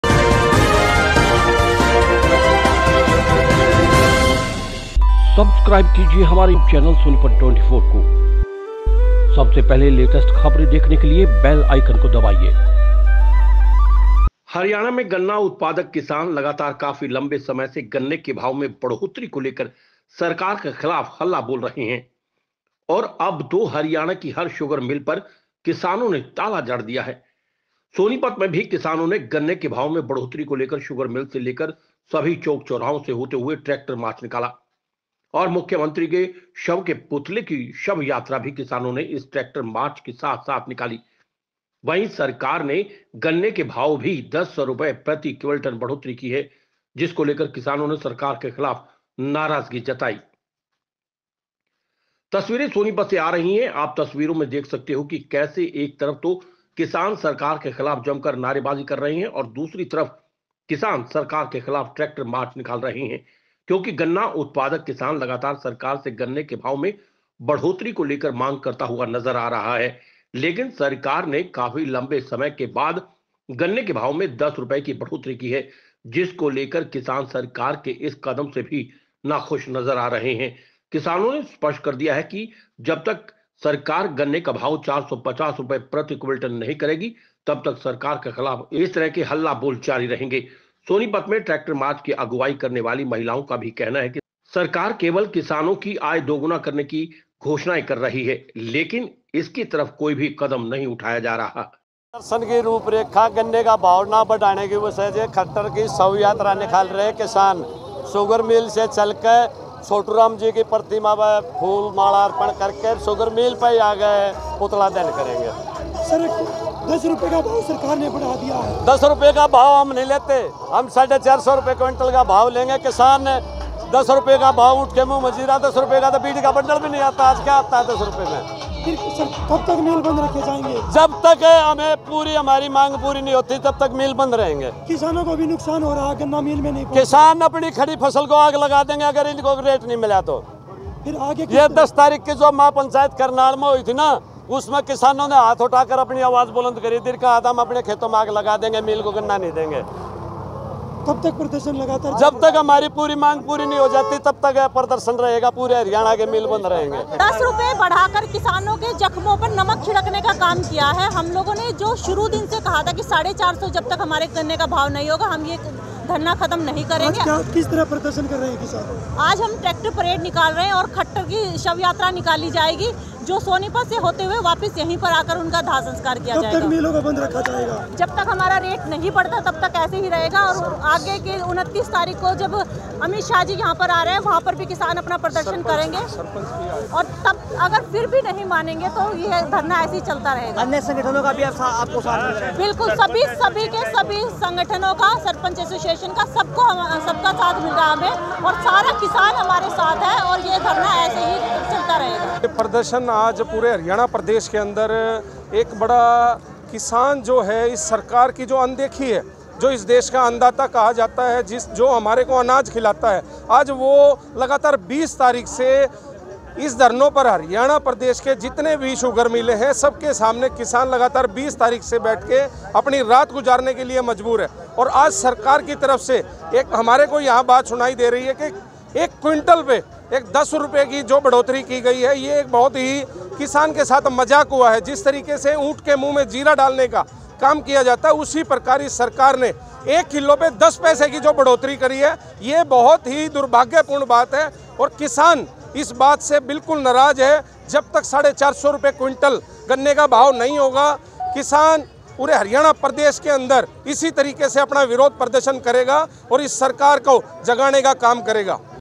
किसानों ने निकाला ट्रैक्टर मार्च, गन्ने पर 10 रुपये प्रति क्विंटल की बढ़ोतरी से नाखुश || Sonipat24
हरियाणा में गन्ना उत्पादक किसानों का सरकार के खिलाफ हल्ला बोल
गन्ने के भाव को 450 रुपए प्रति क्विंटल करवाने के लिए सोनीपत में भी किसानों ने निकाला ट्रैक्टर मार्च
किसानों ने सरकार के खिलाफ की जमकर नारेबाजी
सरकार के 10 रुपये प्रति क्विंटल की बढ़ोतरी से नाखुश नज़र आए किसान

हरियाणा में गन्ना उत्पादक किसान लगातार काफी लंबे समय से गन्ने के भाव में बढ़ोतरी को लेकर सरकार के खिलाफ हल्ला बोल रहे हैं और अब तो हरियाणा की हर शुगर मिल पर किसानों ने ताला जड़ दिया है और आज सोनीपत में भी किसानों ने गन्ने के भाव में बढ़ोतरी को लेकर शुगर मिल से लेकर सभी चौक चौराहों से होते हुए ट्रैक्टर मार्च निकाला और मुख्यमंत्री के शव के पुतले की शव यात्रा भी किसानों ने इस ट्रैक्टर मार्च के साथ साथ निकाली, वहीं सरकार ने आज गन्ने के भाव भी ₹10 प्रति क्विंटल बढ़ोतरी की है जिसको लेकर किसानों ने सरकार के खिलाफ नाराजगी जताई।